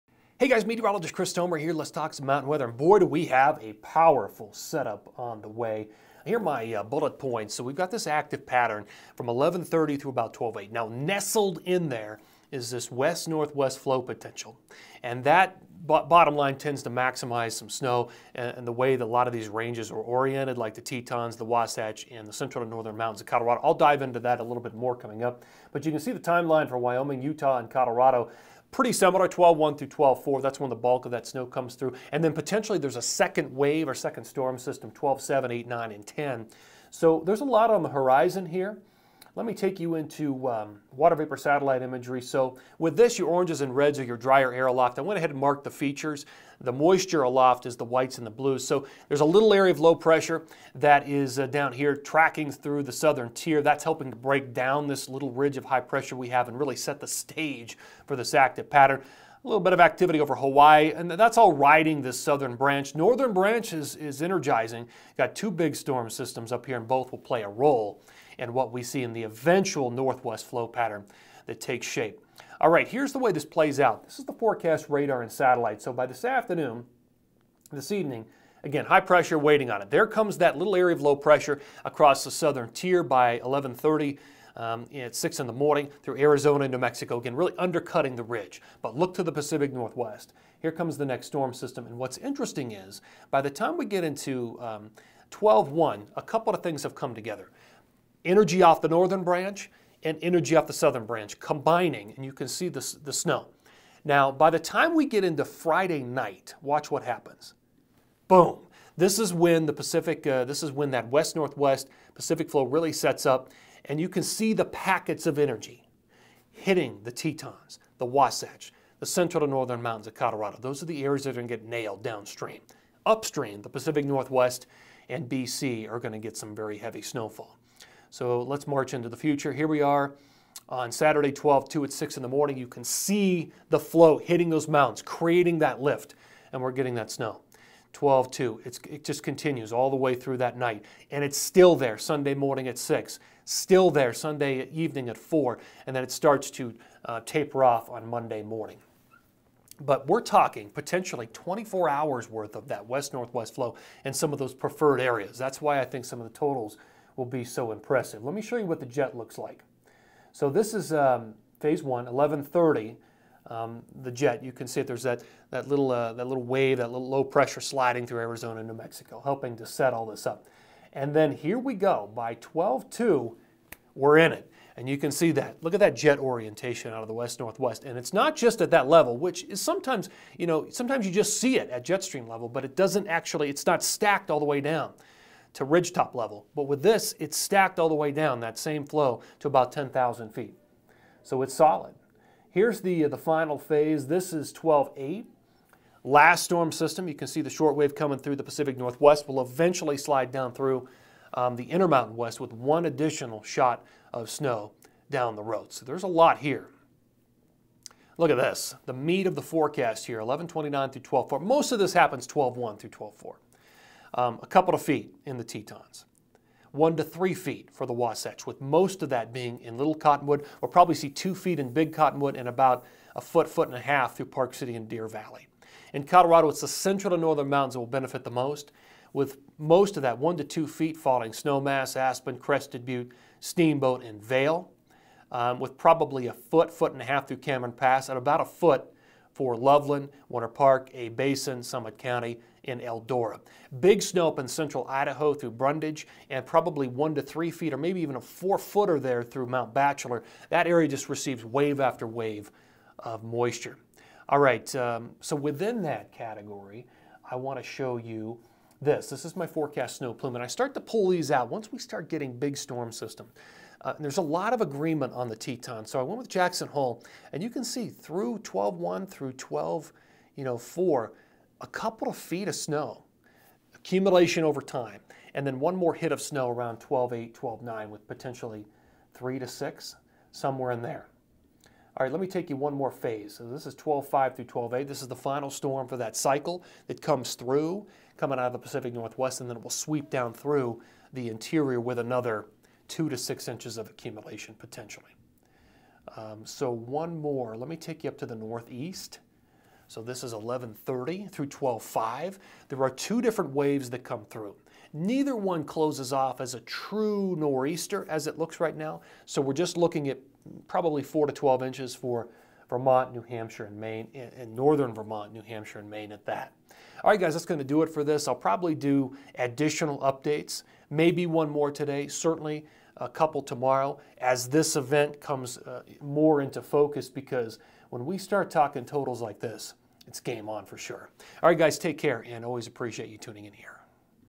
Mountain Weather Update 11/29, Meteorologist Chris Tomer
Powerful WNW Flow potential with big totals in PNW, WY, UT, CO, snow plume, jet pattern through 12/8.